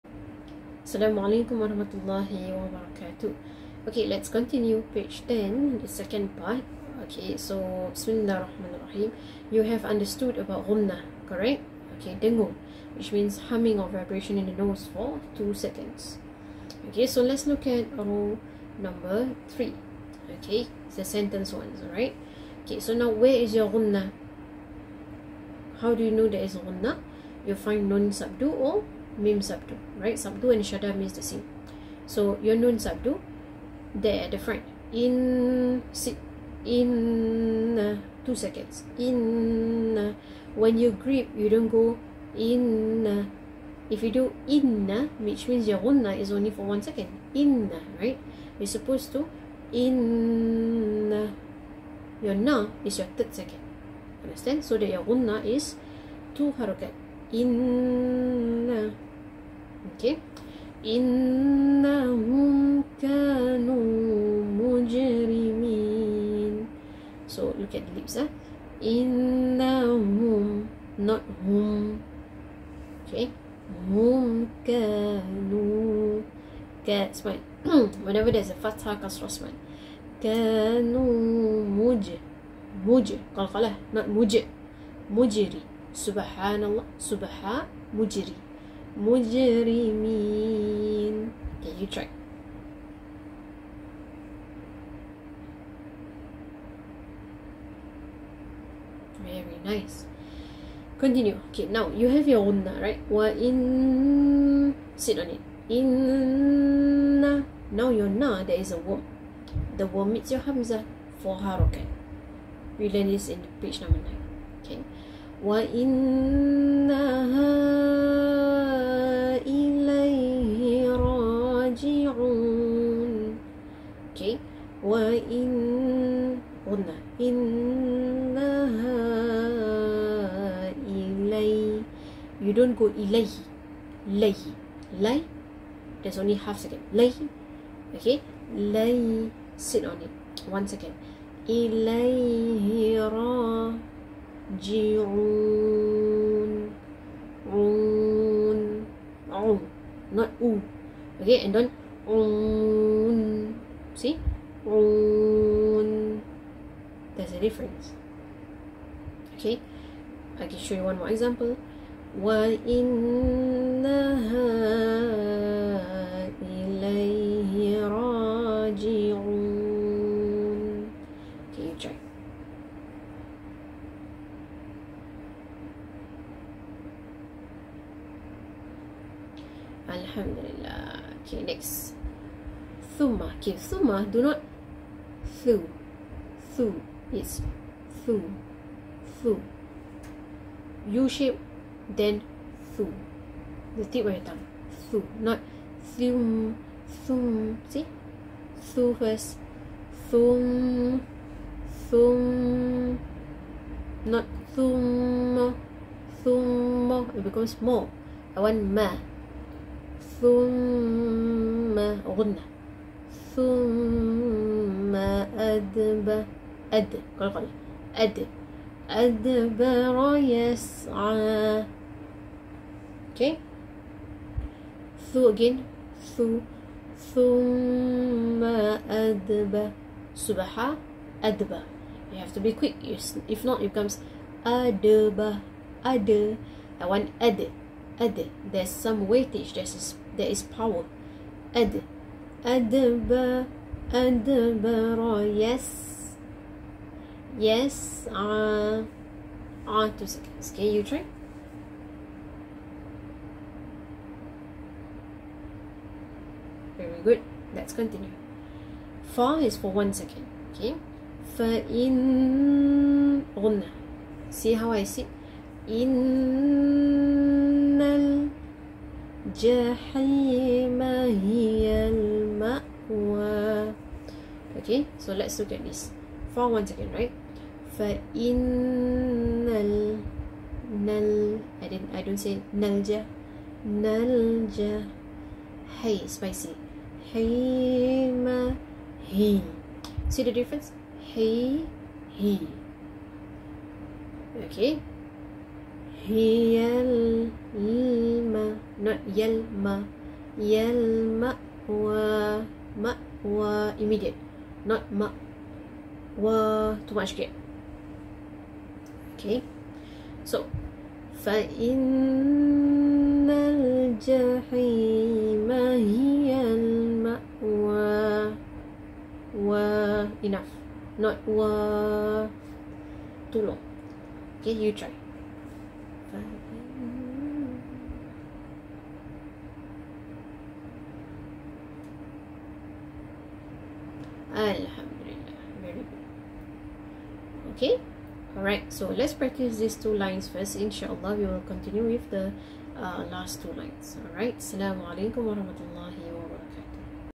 IQRA Book 5 - Page 10 Part 2 (Nun/Mim Shaddah has ghunnah - 2 harakat)
Welcome to Let's Read IQRA Book 5 with Ustazah Rawida.

Look, Listen & Learn together!